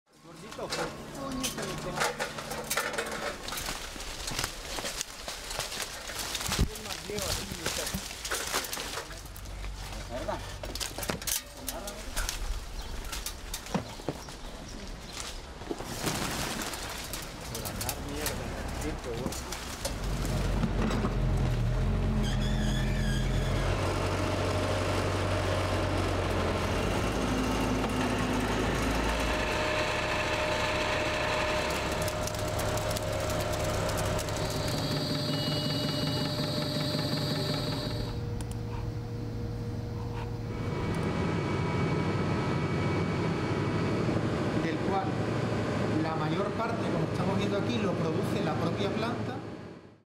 شاهد: في إشبيلية البرتقال المر يُحول إلى كهرباء
طن واحد من البرتقال يمكن أن ينتج ما يصل إلى 50 كيلوواط من الكهرباء، وهو ما يكفي لتزويد خمسة منازل بالطاقة لمدة يوم واحد.